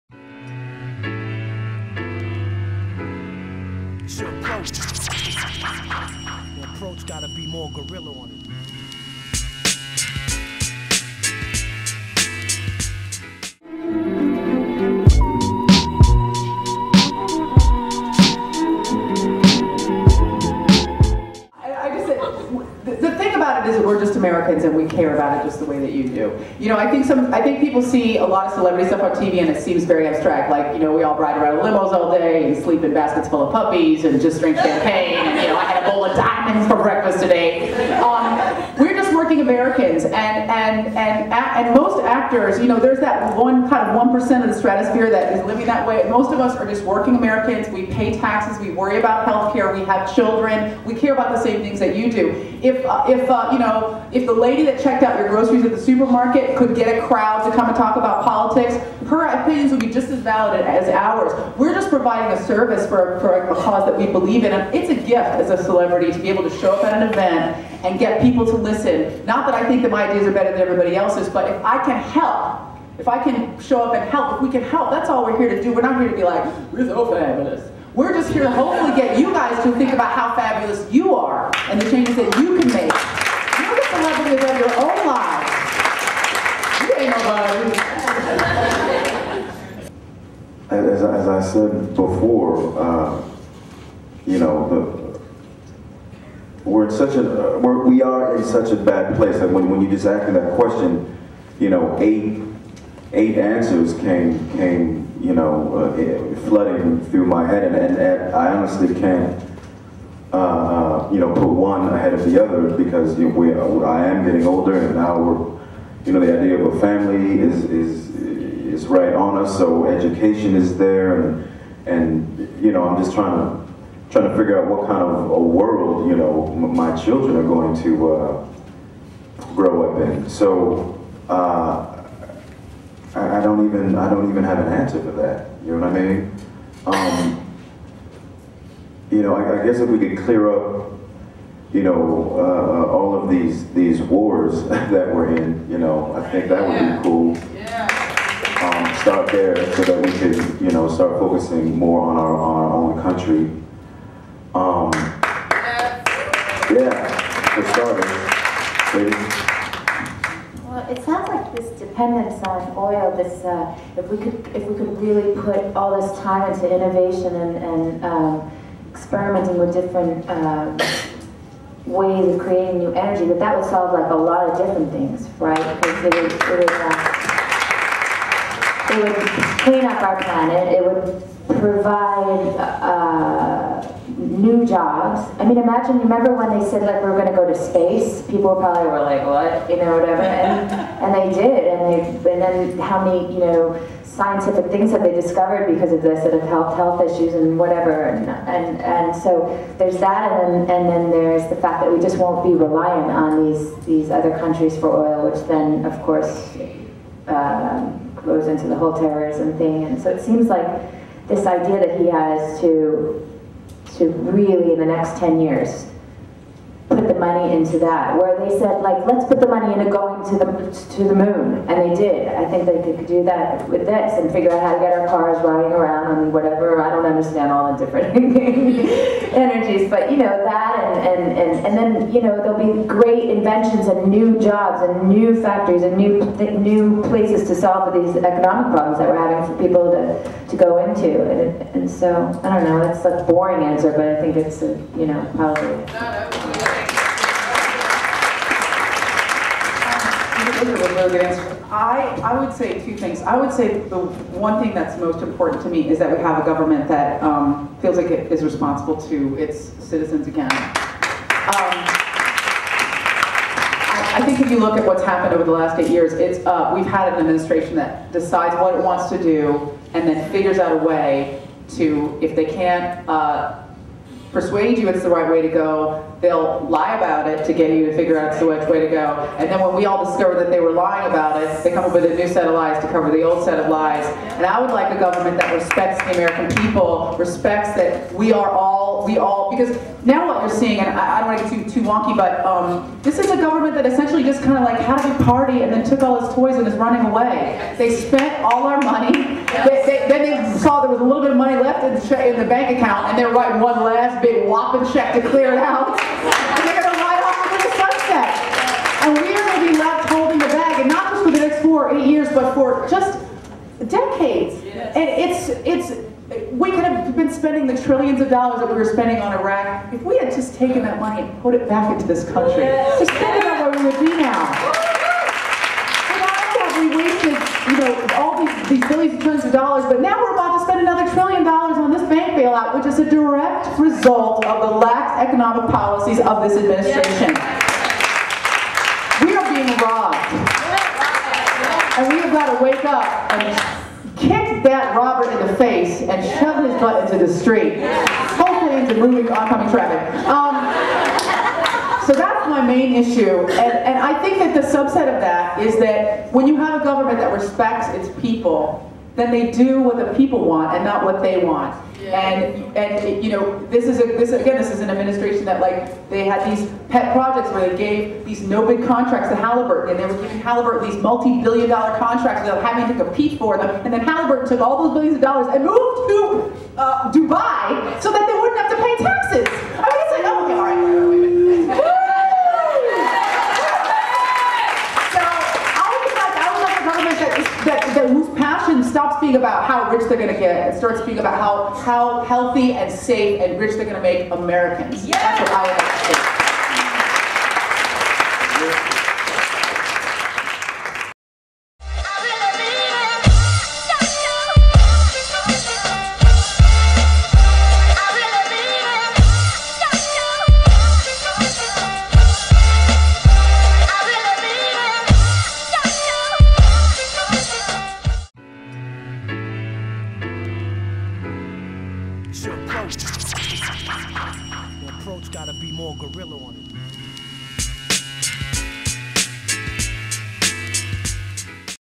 Aisha Tyler, Idina Menzel, Taye Diggs Panel 2
Part 2 of panel discussion on politics: Aisha Tyler, Idina Menzel, and Taye Diggs.